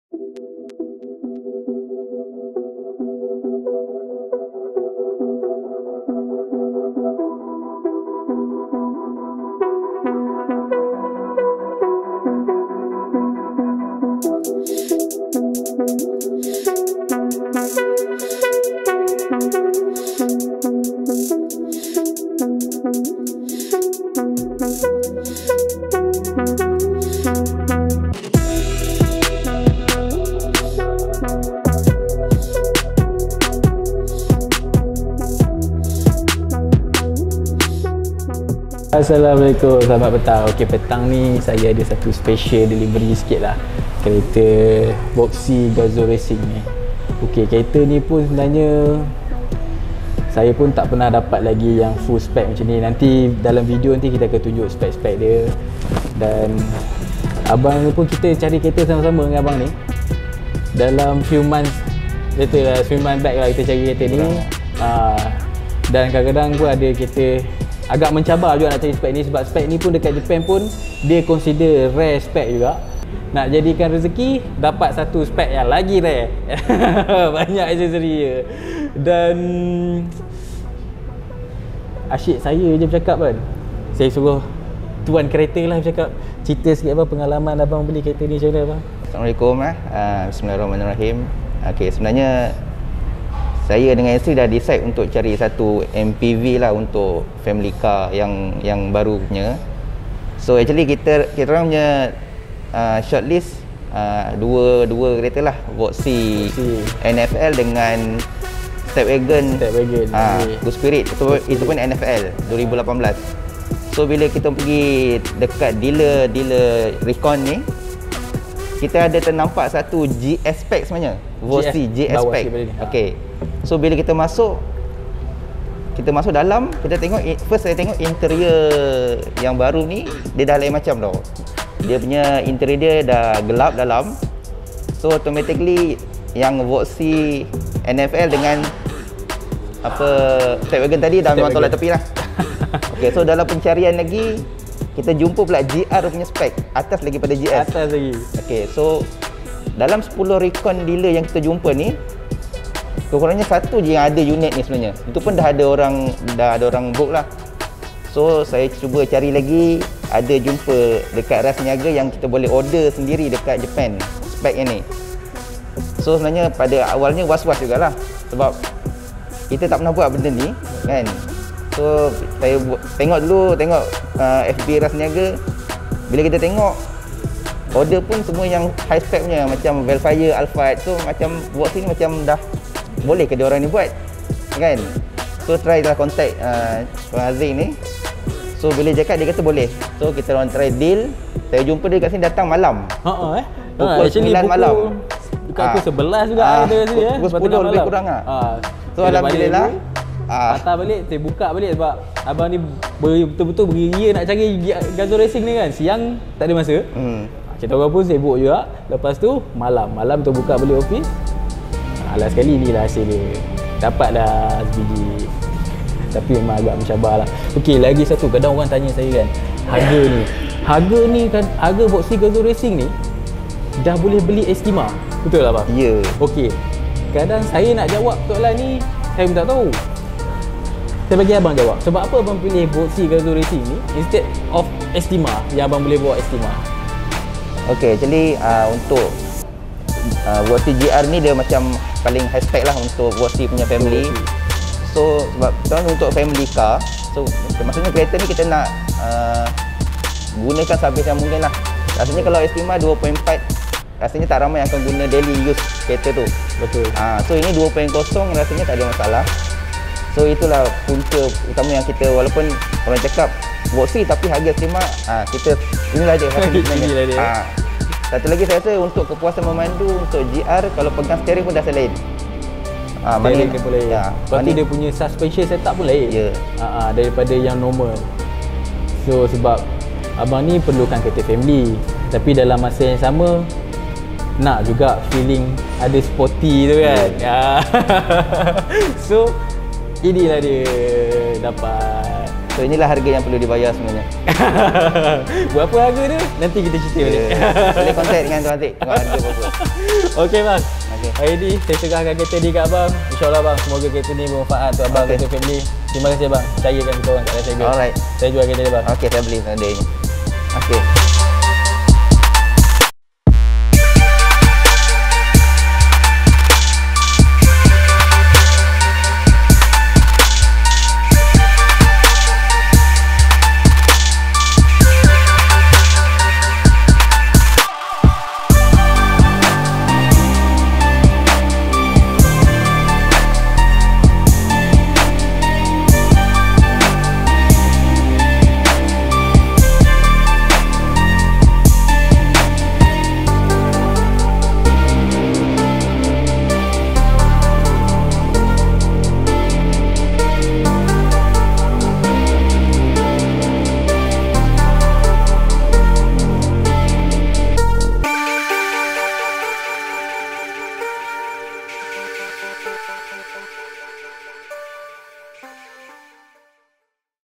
"SEBUAH MPV SPORTY BUAT SI FAMILY" TOYOTA VOXY GR
Alhamdullilah selesai dah sales team kita Haziq menyerahkan kereta kepada customer beliau Tuan Hasnol.. Pertamanya, kami dari pada kumpulan Rasniaga mengucapkan syukur kehadrat Allah S.W.T. kerana dengan limpah kurnianya dapat memudahkan urusan bersama Tuan Hasnol dengan kereta yang beliau inginkan.. TOYOTA VOXY GR!

Tuan Hasnol decide untuk mencari sebuah MPV untuk family dan boleh pandu daily drive.. Yang paling utama bagi beliau ialah nak feeling sporty gitu.. Banyak dah dealer Tuan Hasnol pergi untuk mencari VOXY GR ni.. Tetapi beliau memilih Rasniaga dan membuat direct order terus daripada Japan datang ke Malaysia..

Best kan? 😊

Apa kereta kita nak daripada luar negara yang takda di Malaysia kita terus import je.. Berminat dengan servis yg kami tawarkan?